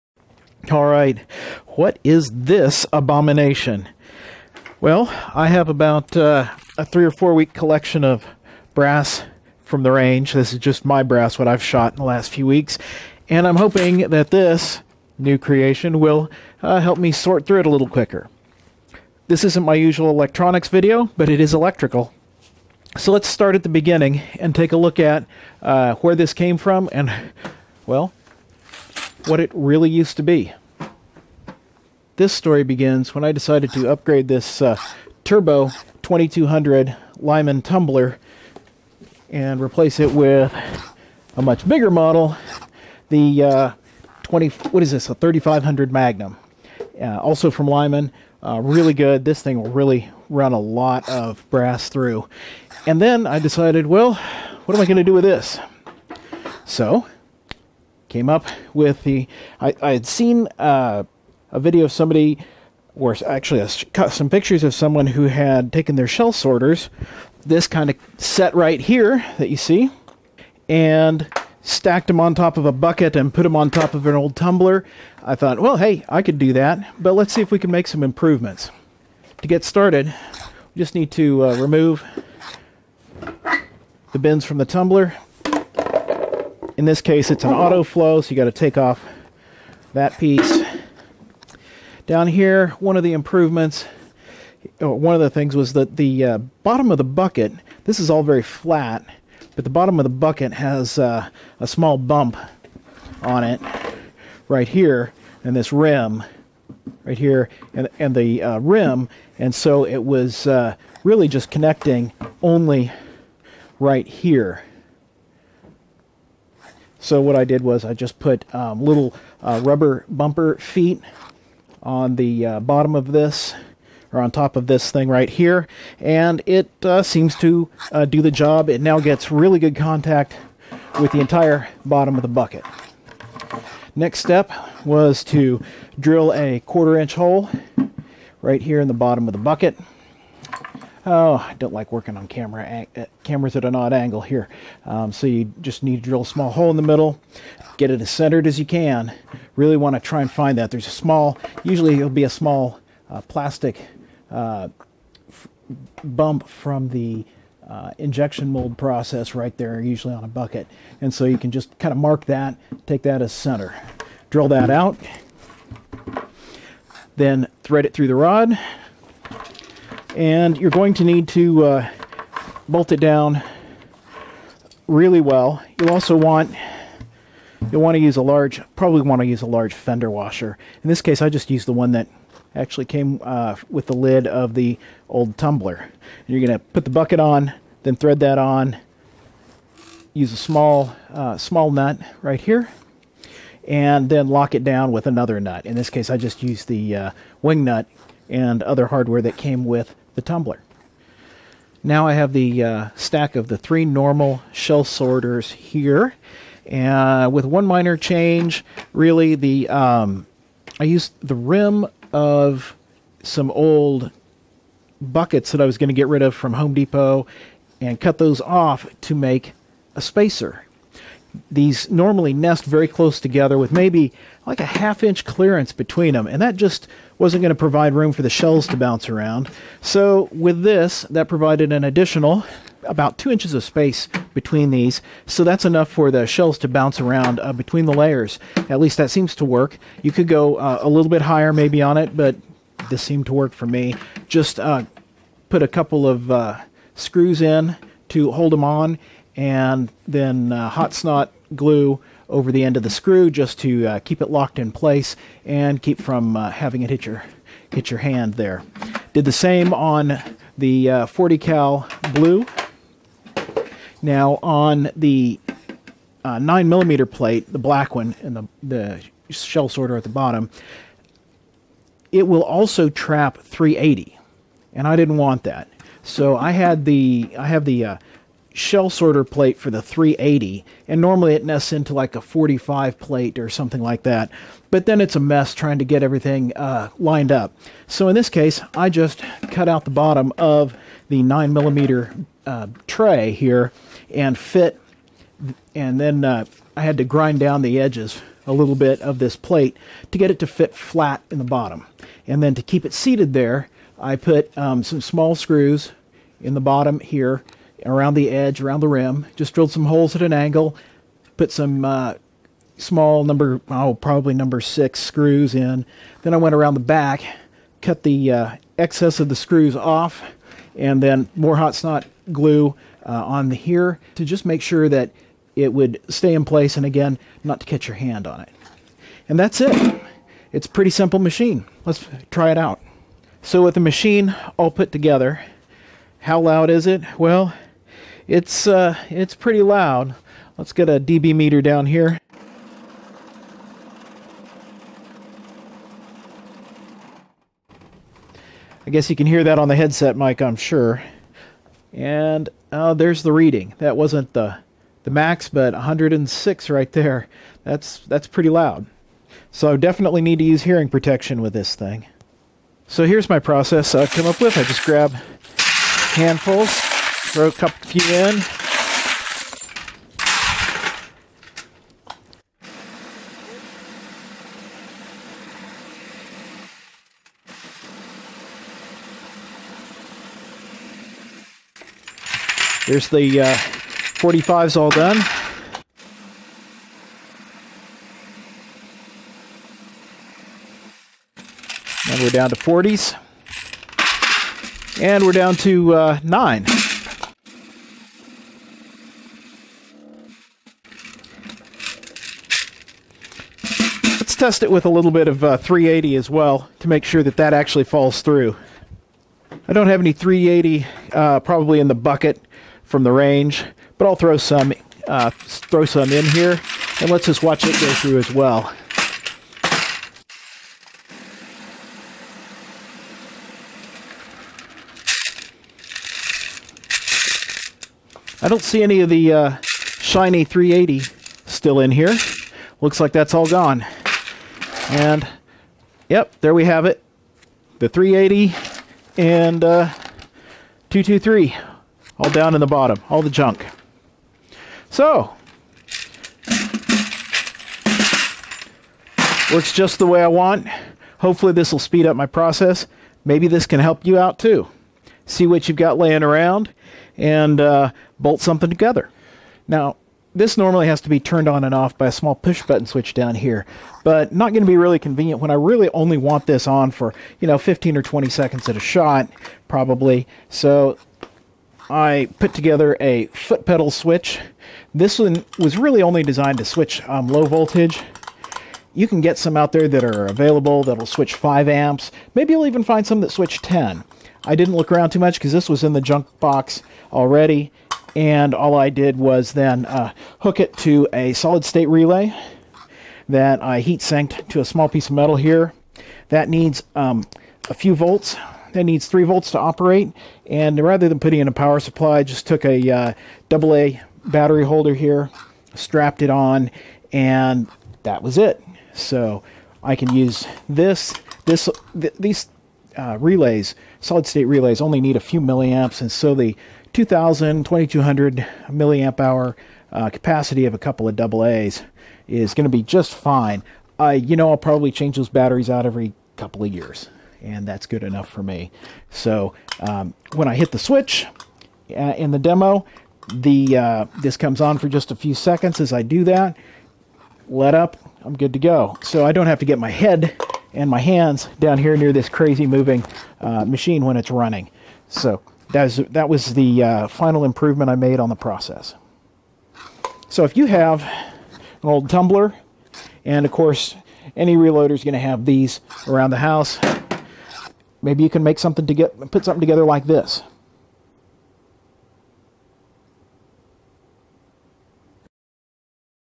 Brass Sorting Machine
A few members from a local shooting club were interested in what I put together for sorting brass.
This isn't my usual electronics video... this is about modifying a vibratory brass tumbler to improve my process for sorting brass cartridge cases by caliber, that were picked up at a shooting range.

This shows how the tumbler and existing sorting trays were modified to cut down on the manual work.  Instead of laboriously shaking brass though each tray one at a time, this allows the user to pour unsorted brass into the top and have it vibrate through the various layers.

Let me know if you have any enhancements that you have come up with on this labor intensive task!
The items in this video are a Lyman 2200 tumber, and a set of sorting trays.  You can get these items from many sources, but here are some links.